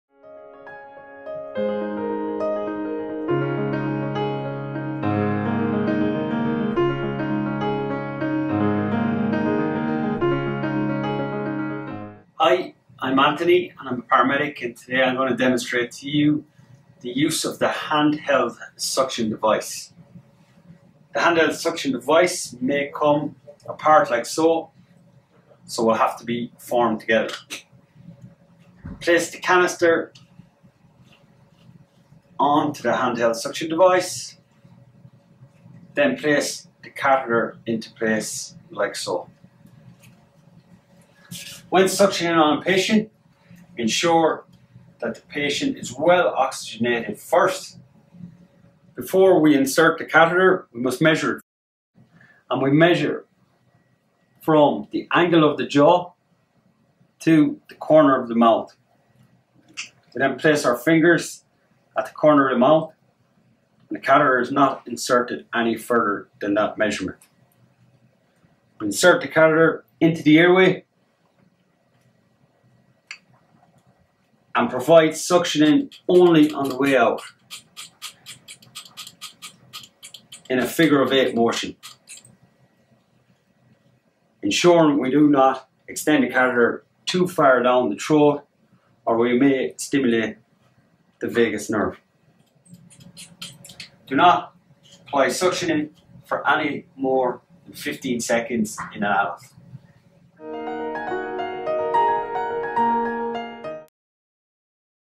Use of a Manual Suction Device
This video is part of a new series of basic clinical skills videos.

It describes the use of a manual suction device for creating a vacuum and removing secretions or bodily fluids in any clinical situation.

This video demonstrate the use of the manual suction device only and does not cover all indications and contraindications to the use of such a device.